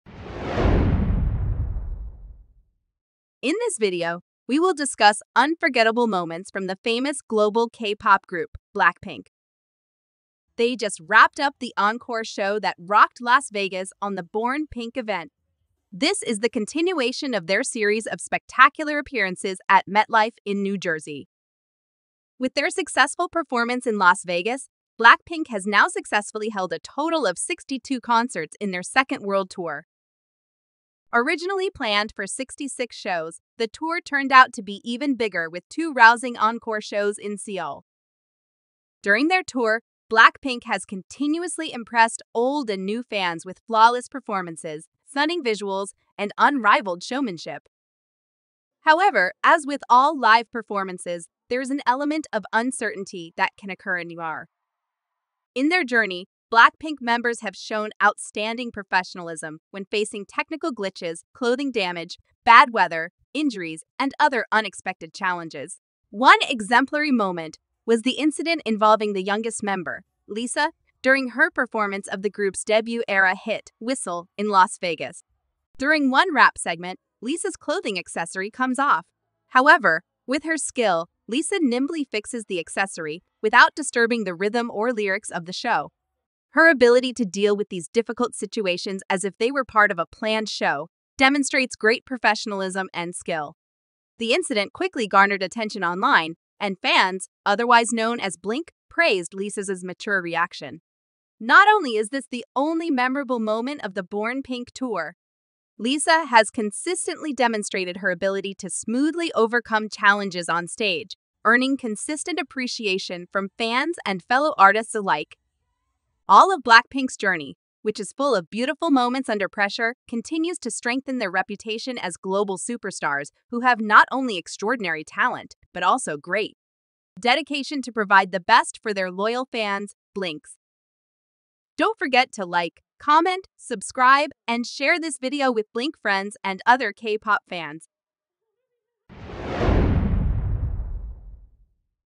Blackpink Stuns on the Encore Stage in Las Vegas
Blackpink just finished the encore show that rocked Las Vegas on the show "Born Pink". This is the continuation of their series of spectacular appearances at MetLife in New Jersey.